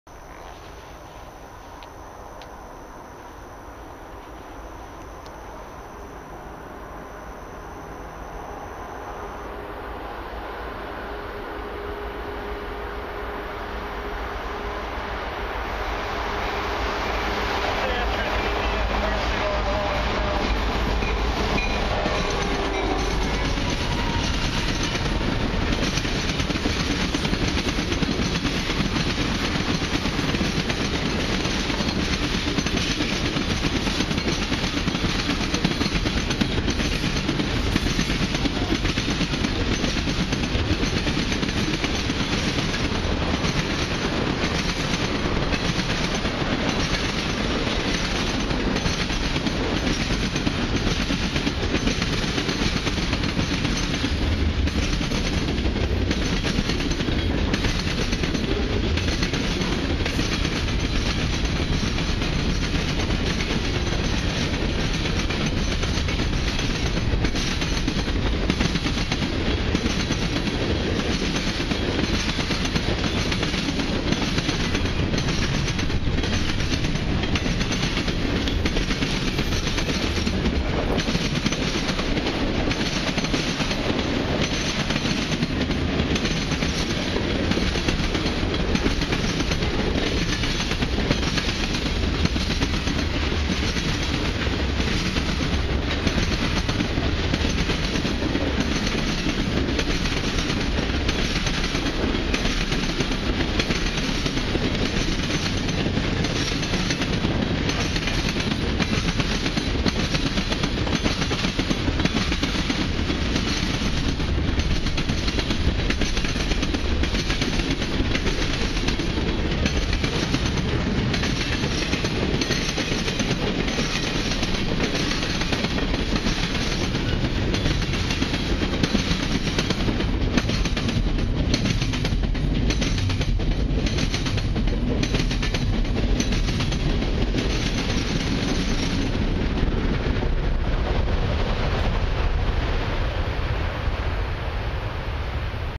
Dusk Shot/CN 316 w/ CN Engines 5653 & 2163 (ex BNSF 803) lead this long SB mixed freight train
.in Severn Bridge, Ontario.

here's my 4th catch from vacation and 4th and final catch from the Sunday.

check out the Herzog stuff at the end of the train. was pretty cool to see as i have never seen that kind of Herzog stuff on the Bala Sub. ive seen it tho on the CP Galt Sub. CP 242 had it once at the end of the train.

video shot between the MP's of 93.80 & 94 on the CN Bala Sub.

& video taken- Sunday July 8th 2012 at 8:41pm
and as always, Comment & Enjoy !
thanks ! =]